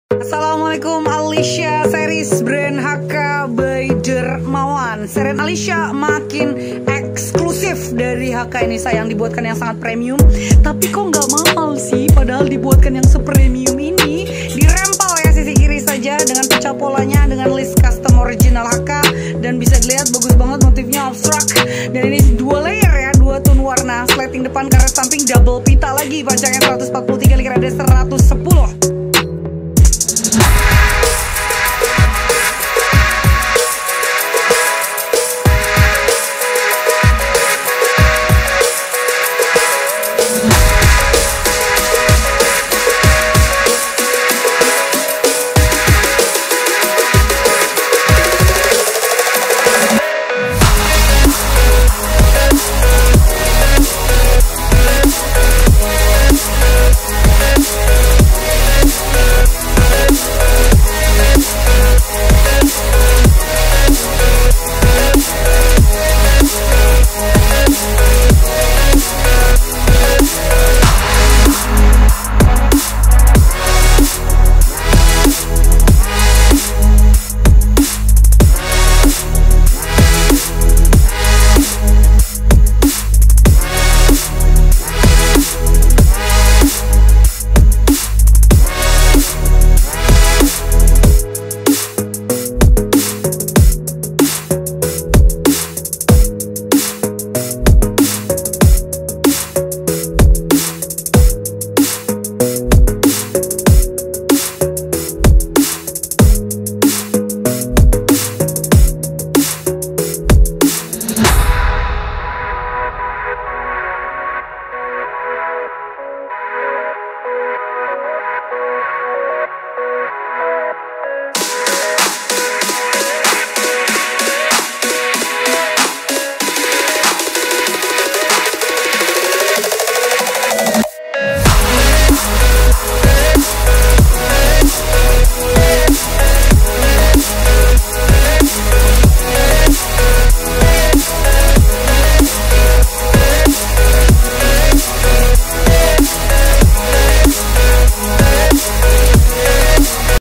ALISYA SYAR'I THE SERIES by HK DERMAWAN
Readyyy readyyy 😍🥰

ALISYA SYAR'I THE SERIES

Bahan :Ceruty baby doll premium mix ceruty baby doll printing
Khimar 2 layer
Ld :110 cm
Pb :143
All size

*Ada karet dan tali di bagian samping pinggang

*Busui wudhu friendly
Di percantik dengan swarosky

*Kita punya do'a kita punya ALLAH SWT🥺🤲🙏😇